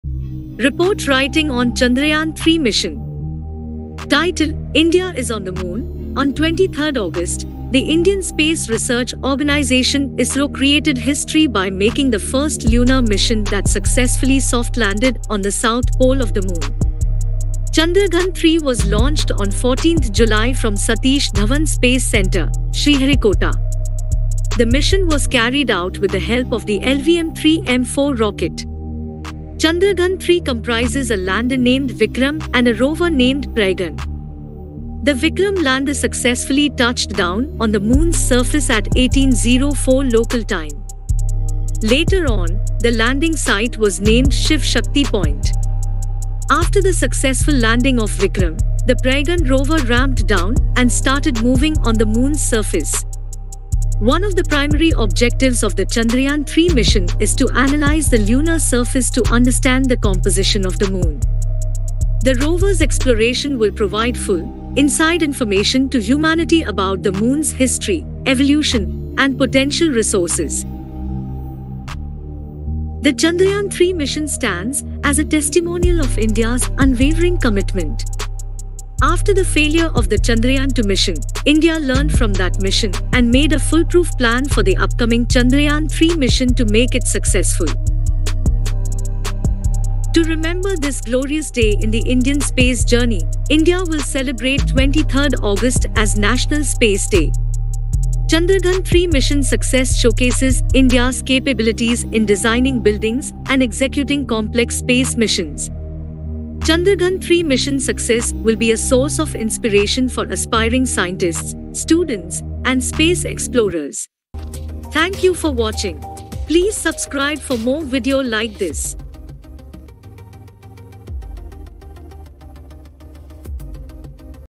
Report Writing on Chandrayaan 3 Mission
Report Writing on Chandrayaan 3 Mission brief The Indian Space Research Organisation (ISRO) has once again made a significant contribution to lunar exploration with the successful launch and landing of the Chandrayaan-3 mission. This mission, which is the third in ISRO's Chandrayaan programme, comprises a lander named Vikram and a rover named Pragyan.